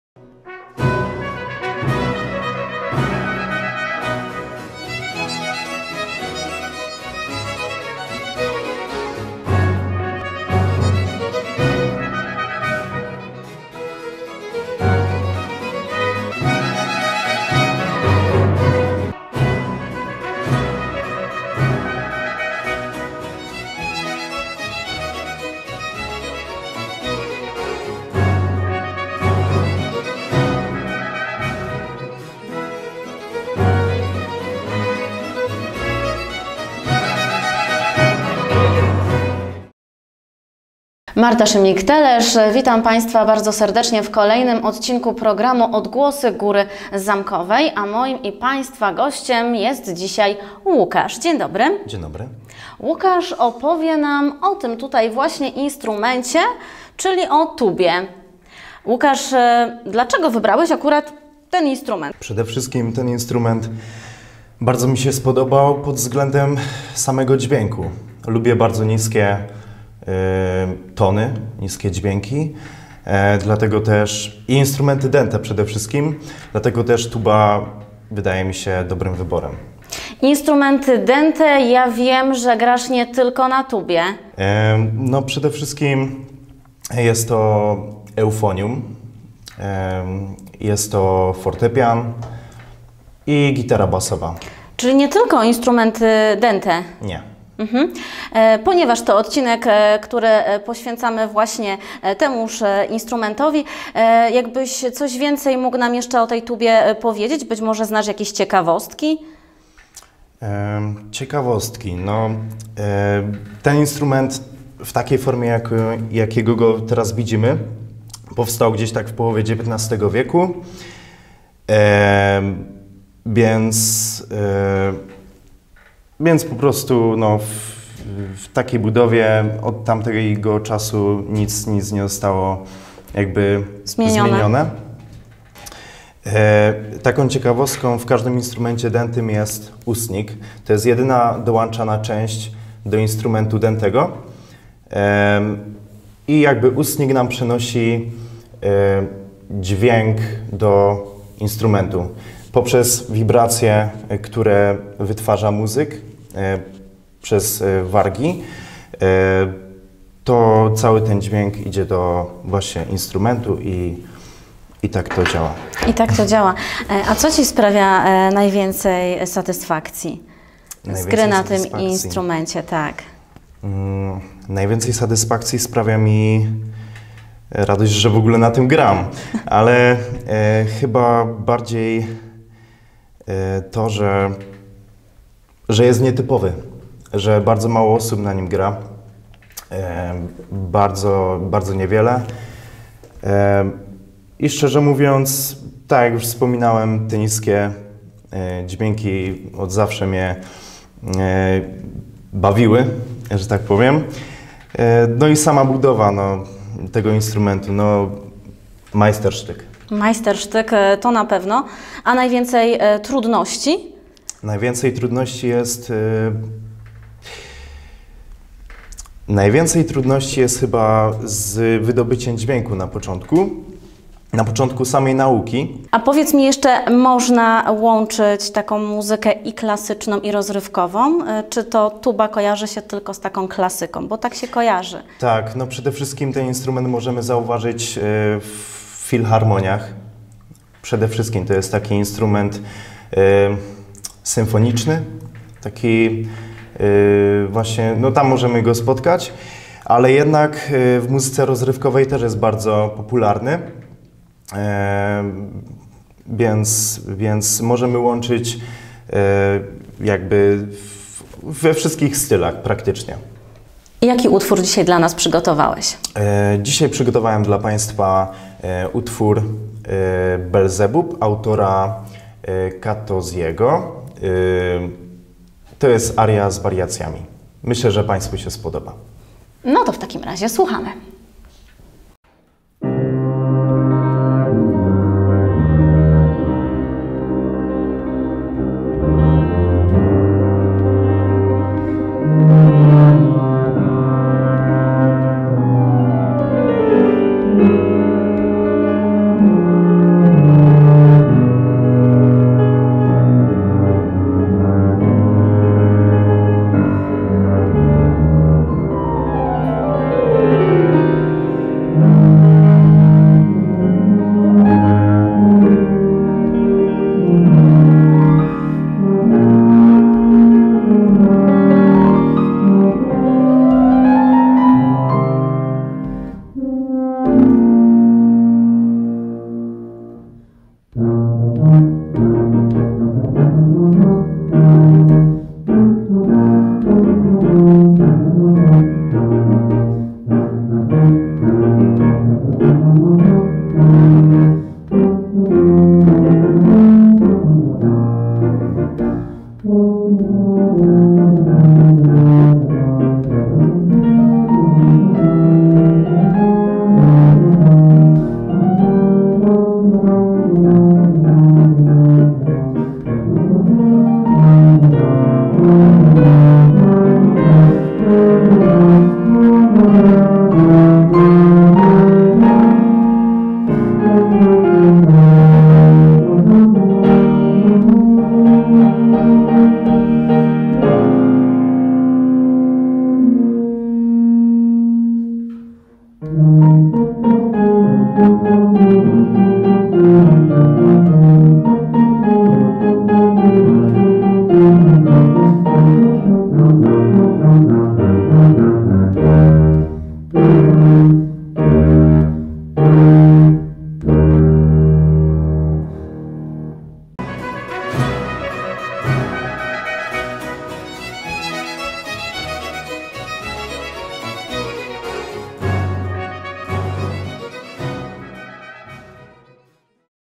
Odgłosy Góry Zamkowej - Tuba (odcinek15)
Odgłosy Góry Zamkowej to program OX.PL realizowany wspólnie z Państwową Szkołą Muzyczną I i II stopnia w Cieszynie. Emisja w każdą niedzielę. Dzisiaj (06.03) zapraszamy do obejrzenia odcinka, którego odgłosem będzie TUBA. Instrument zaprezentuje i zagra dla nas uczeń PSM w Cieszynie Łukasz Brudny. Akompaniator mgr Joanna Lipowczan-Stawarz.